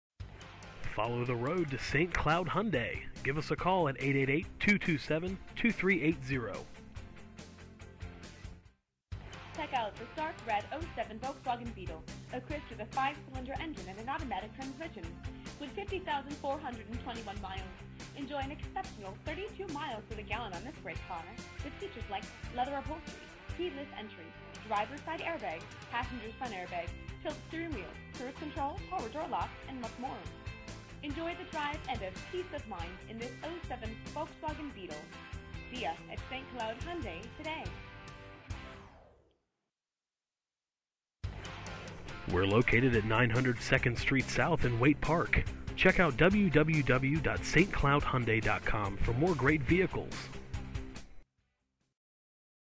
2007 Volkswagen Beetle ST. Cloud Hyundai Waite Park, MN
2007 Volkswagen Beetle- Check out this dark Red 2007 Volkswagen Beetle, equipped with a 5 Cyl. engine and an automatic transmission with 50,421 miles. Enjoy an exceptional 32 miles to the gallon on this great car with features like Leather Upholstery, Keyless Entry, Driver Side Air Bag, Passenger's Front Airbag, Tilt Steering Wheel, Cruise Control, Power Door Locks, and much more. Enjoy the drive and have peace of mind in this 2007 Volkswagen Beetle. See us at ST. Cloud Hyundai today!
Miles-  50,421
VIN- 3VWSW31CX7M515917

ST. Cloud Hyundai
900 2nd Street South
Waite Park, Minnesota 56387